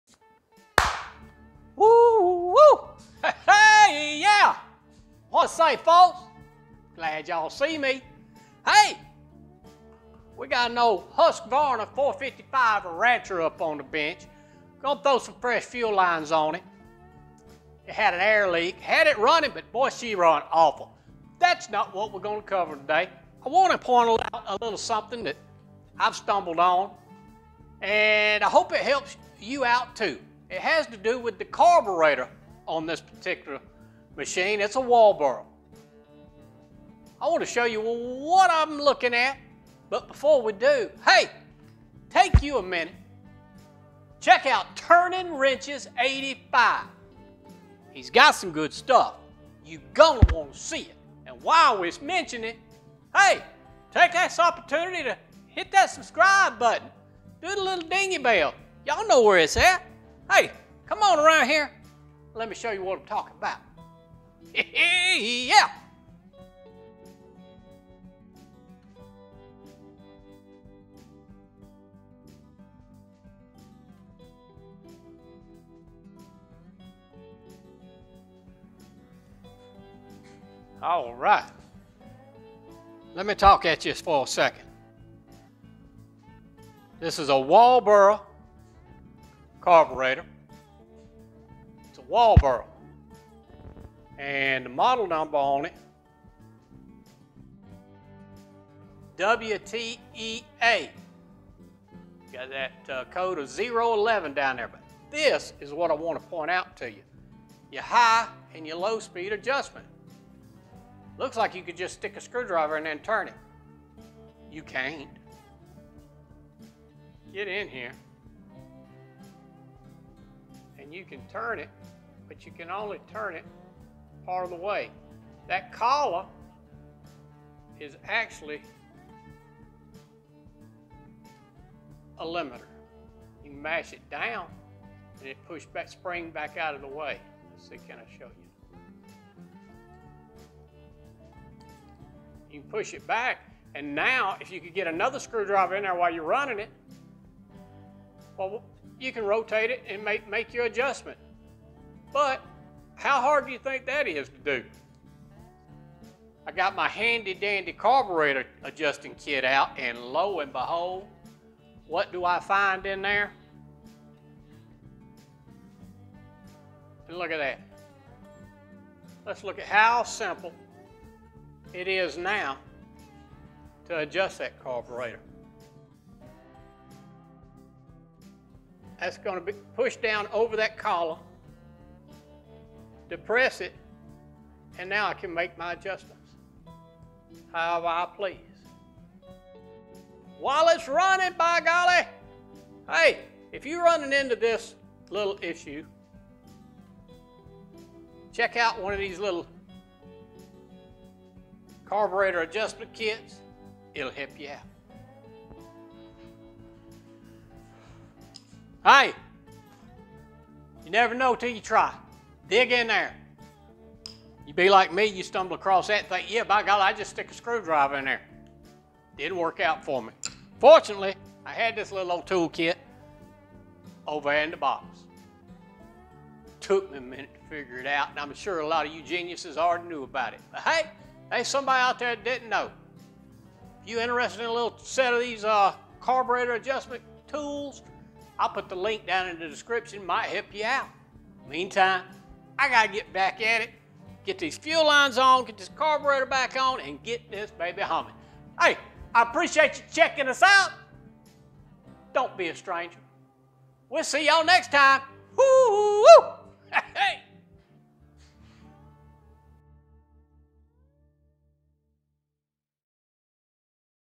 Carburetor Adjustment, Husqvarna 455 Rancher Chainsaw
Check out how this Carburetor Adjustment on a Husqvarna 455 Rancher Chainsaw works.

Carburetor Adjusting Kit